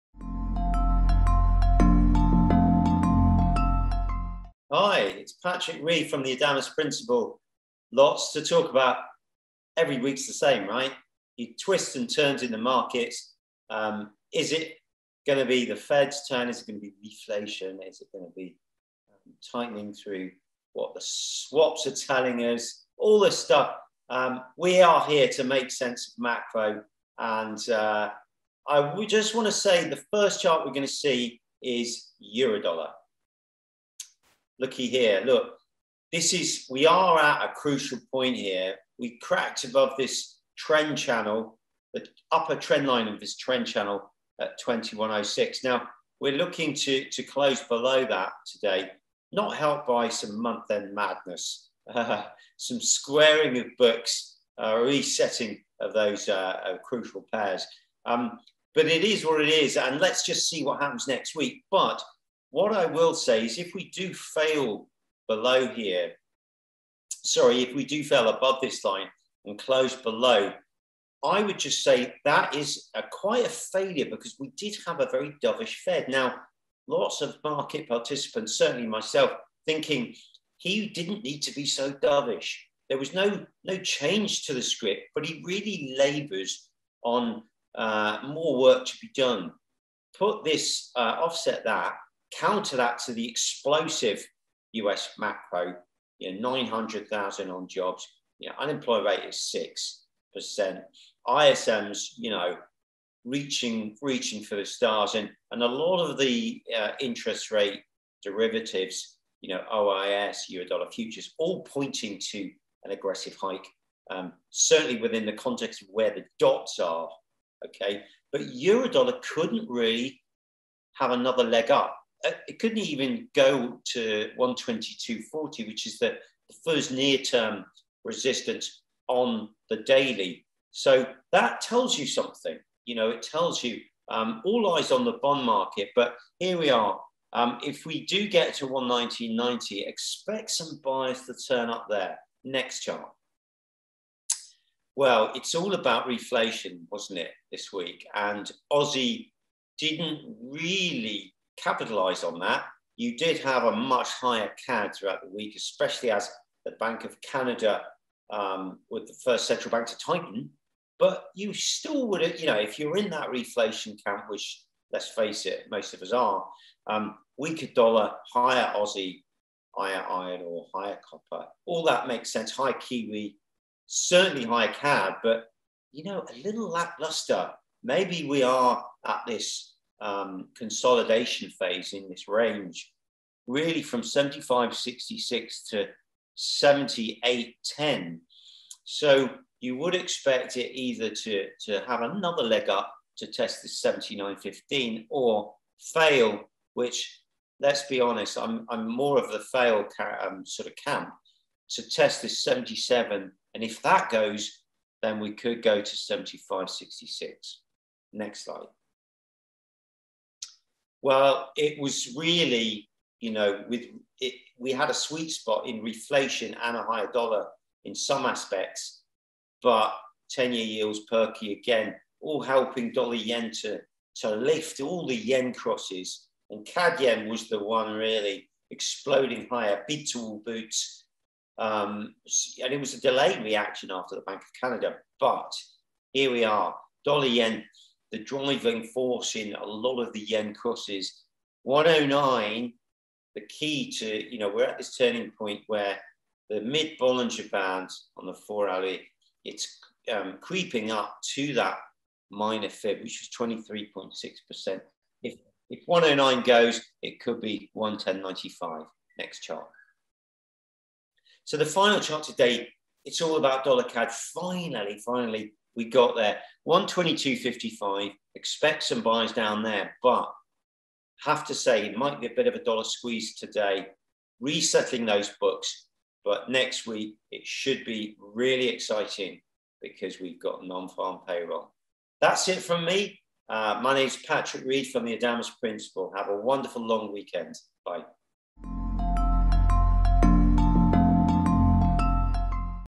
Macro and Currencies
Patrick takes a technical look at a range of currency pairs. Macro always impacts FX, but how has it done so this week?